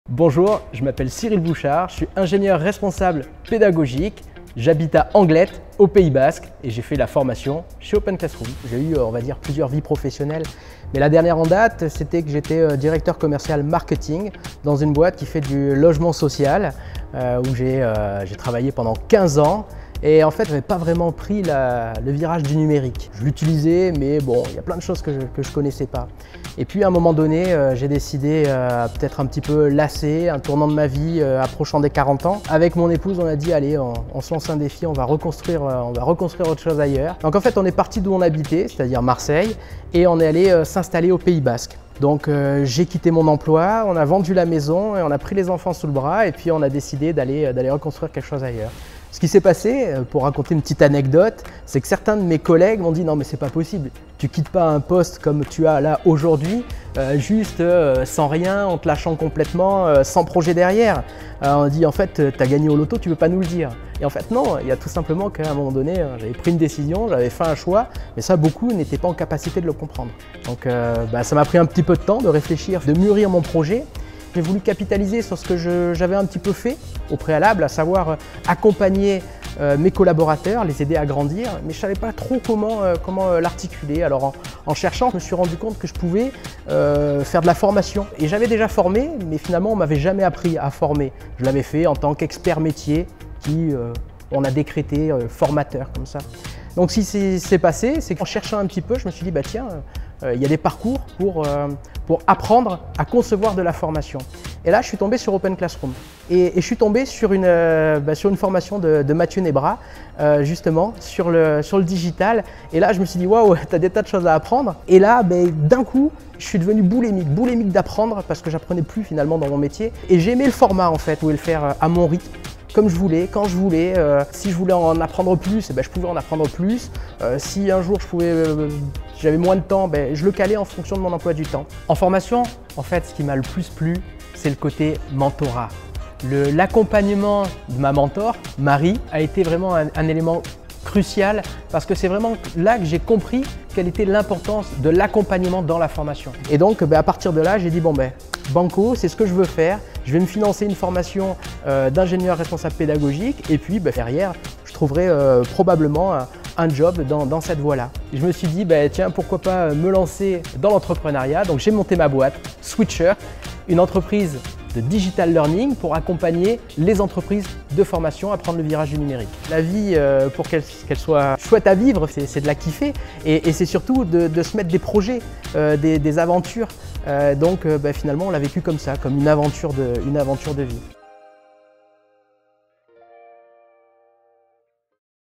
Cyril, ancien de la formation Ingénieur Responsable Pédagogique
Cyril a monté son entreprise de conseil en formation numérique, Sweetcher Digital, au terme de sa formation. Une aventure qu'il n'aurait jamais soupçonnée avant que la formation OpenClassrooms lui ouvre des possibles.